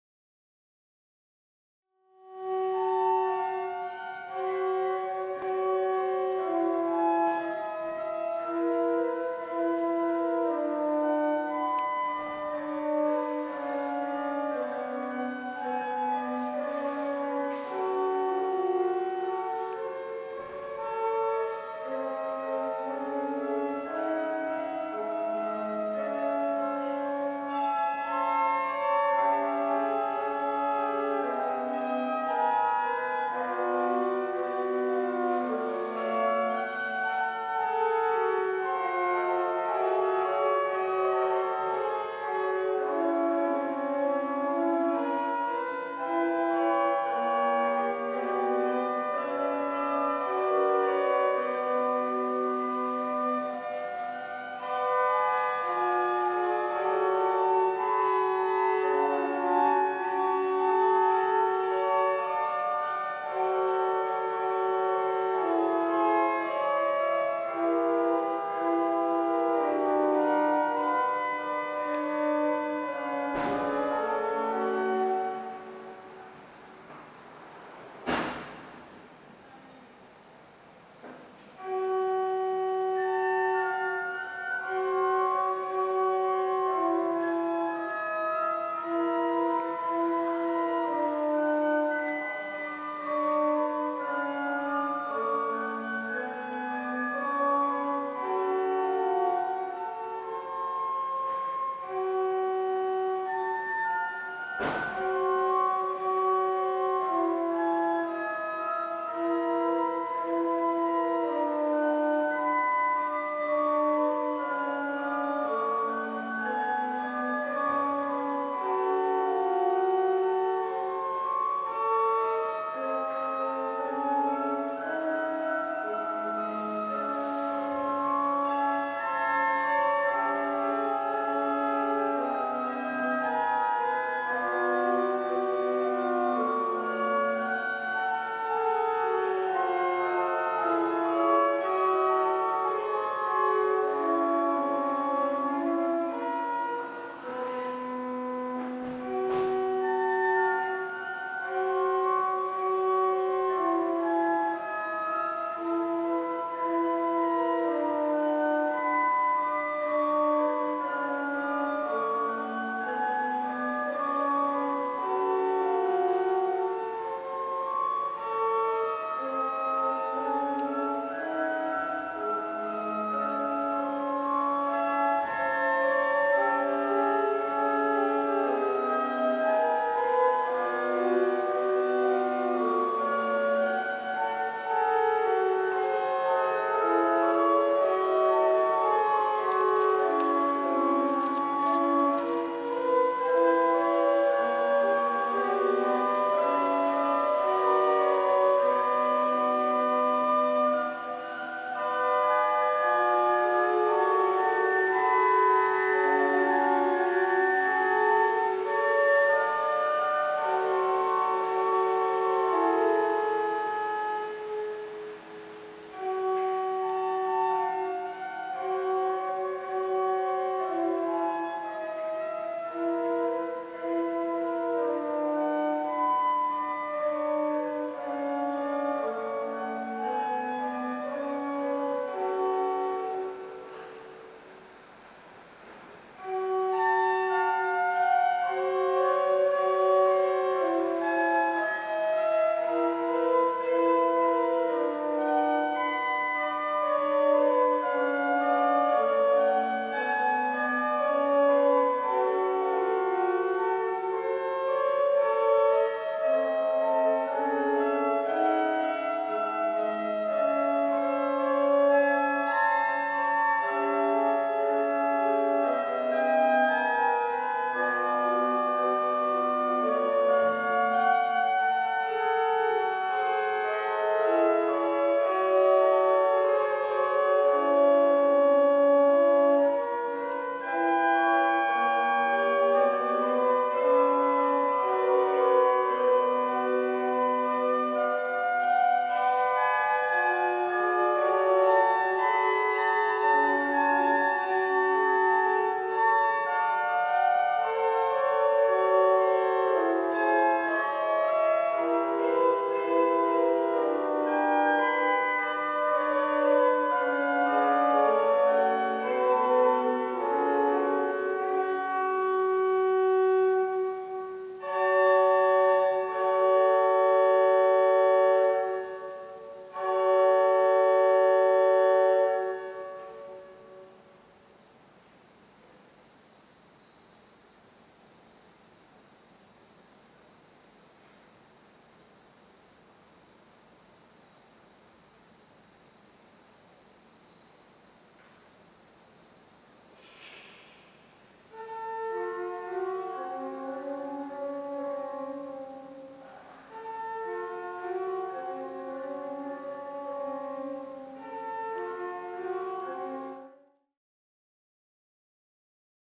目黒教会 礼拝堂
目黒教会　パイプオルガンが流れています。